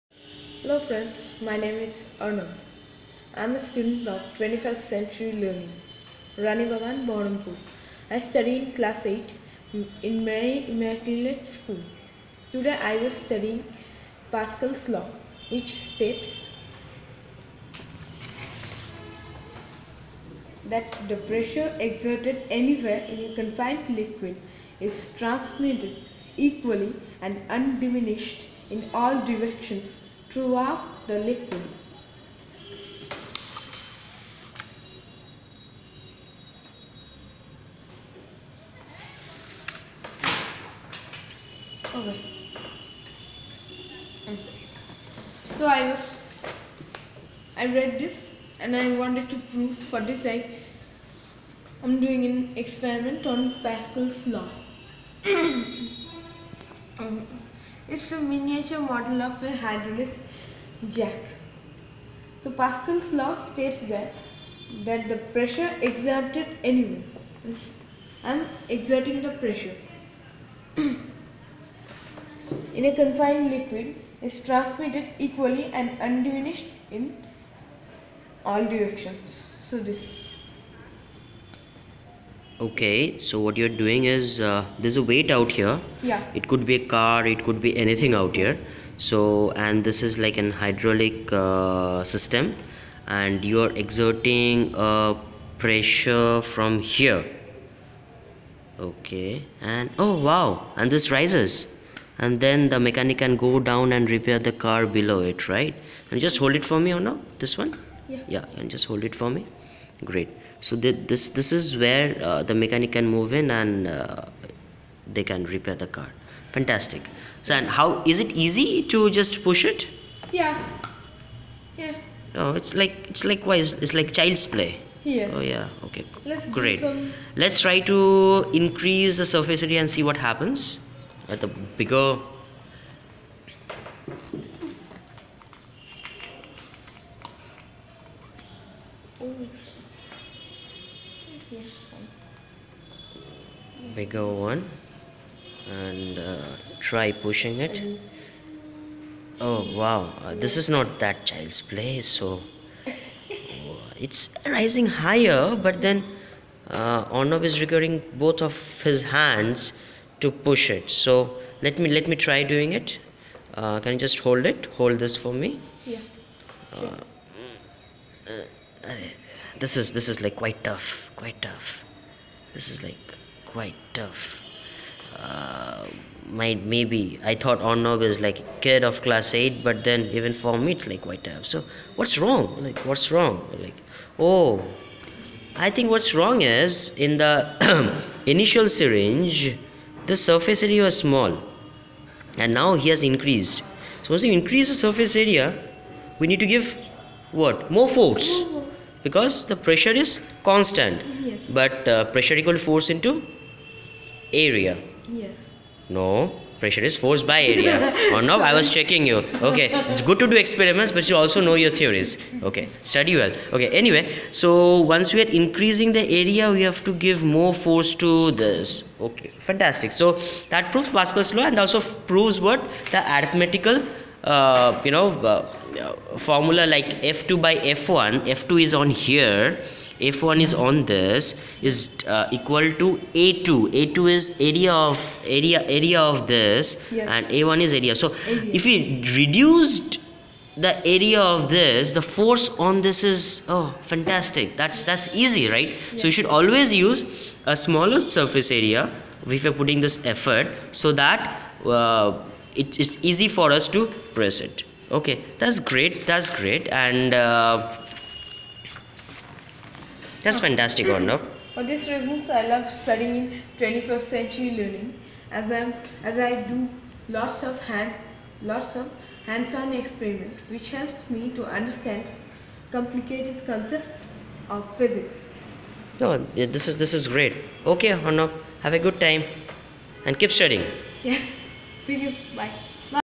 Pascal's law made easy. Pressure in Fluids
Pascal's law very easily explained via experiment .A must watch for all students